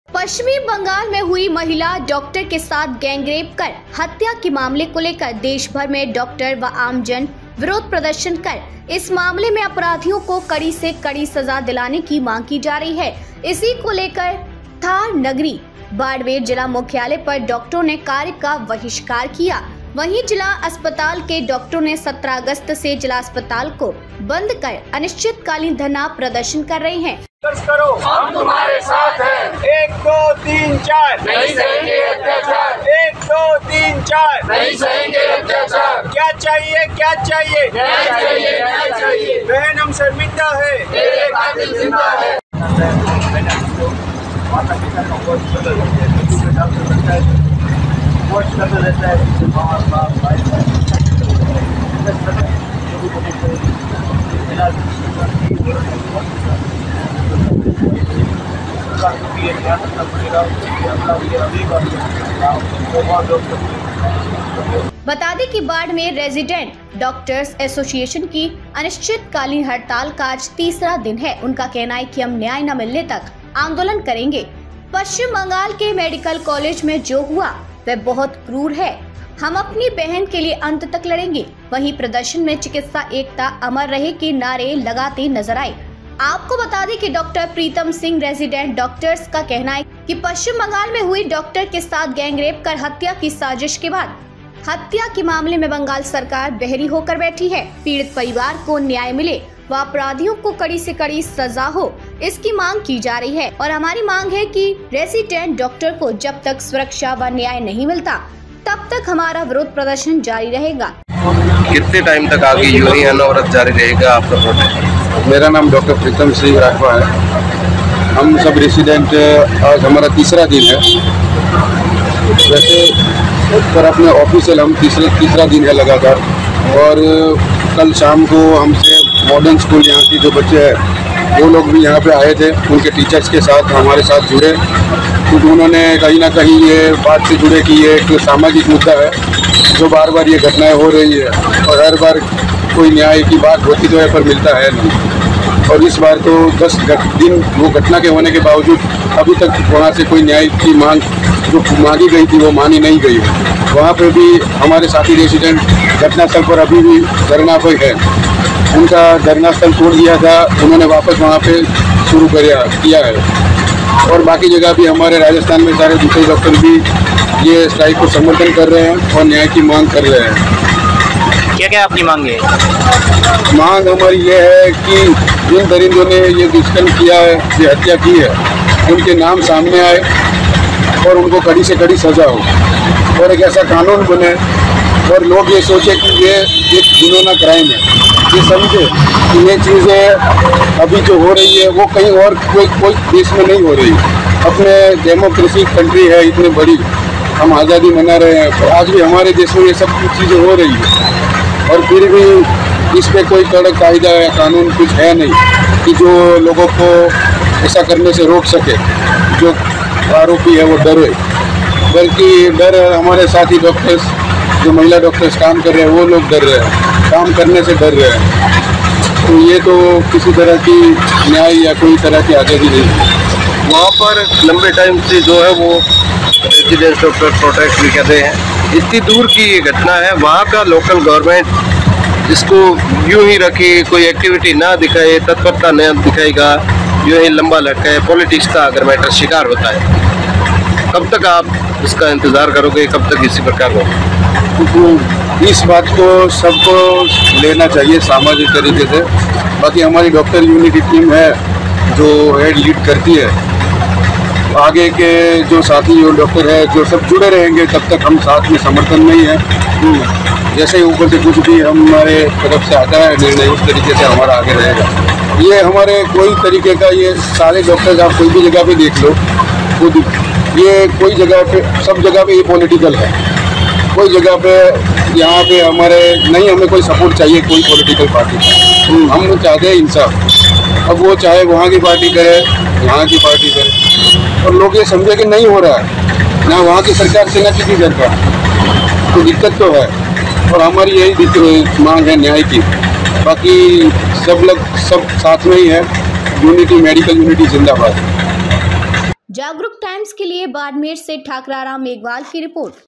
Barmer: महिला Doctor के साथ गैंगरेप कर हत्या के मामले को लेकर देश भर में लगातार विरोध प्रदर्शन जारी
महिला डॉक्टर के साथ गैंगरेप कर हत्या के मामले को लेकर देश भर में लगातार विरोध प्रदर्शन जारी

पश्चिमी बंगाल में हुई महिला डॉक्टर के साथ गैंगरेप कर हत्या के मामले को लेकर देश भर में डॉक्टर व आमजन विरोध प्रदर्शन कर इस मामले में अपराधियों को कड़ी से कड़ी सजा दिलाने की मांग की जा रही है, इसी को लेकर थार नगरी बाड़मेर जिला मुख्यालय पर डॉक्टरों  ने कार्य का बहिष्कार किया।

वहीं जिला अस्पताल के डॉक्टरों ने 17 अगस्त से जिला अस्पताल को बंद कर अनिश्चितकालीन धरना प्रदर्शन कर रहे हैं, बता दे बाड़मेर रेजिडेंट डॉक्टर्स एसोसिएशन की अनिश्चितकालीन हड़ताल का आज तीसरा दिन है। उनका कहना है हम न्याय न मिलने तक आंदोलन करेंगे। पश्चिम बंगाल के मेडिकल कॉलेज में जो हुआ वह बहुत क्रूर है हम अपनी बहन के लिए अंत तक लड़ेंगे। वही प्रदर्शन में चिकित्सा एकता अमर रहे, के नारे लगाते नजर आए।